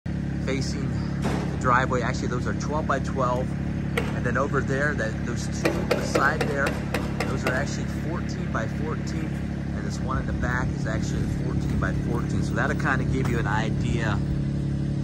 “Overhead Door Framing”…building out…the bays ⚙️
50x80 pole barn building under construction.